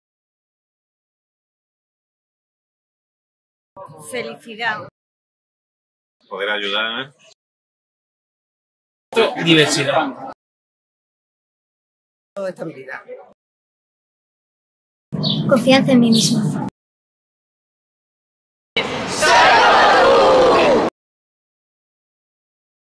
Voluntariado y empoderamiento Fundación SOYCOMOTU
Un equipo de más de cien voluntarios con y sin problemas de salud mental luchando contra el estigma, facilitando espacios de inclusión y participación social, educando y promocionando hábitos para la salud y el bienestar emocional en centros educativos de primaria y secundaria, generando acciones artísticas, reivindicativas y formativas para empoderar y estimular la defensa de los derechos de las personas con diversidad en salud mental y/o neurodivergencia.